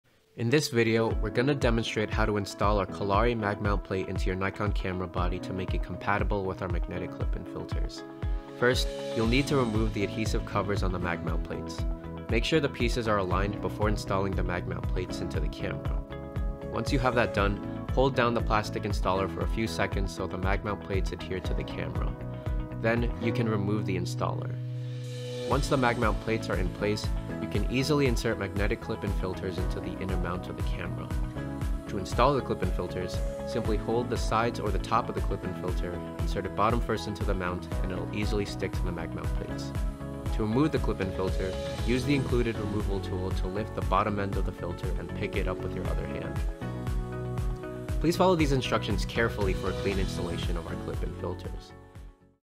Kolari Z-Mount Mag Mount Plate Installation for Nikon Z6, Z7, Z5, & Z9
Introducing our new Z-mount clip filters! Compatible with the Nikon cameras Z-mount lenses, these can be used to add rear filtering to Nikon Z-mount cameras. The clip filter can also be used to quickly change the effective spectrum of a full spectrum camera or stock camera, bringing it from visible light to infrared, or other.

The Mag Mount plate allows our clip-in filters to be compatible with Nikon full-frame mirrorless cameras (Nikon Z6, Z7, Z5, & Z9).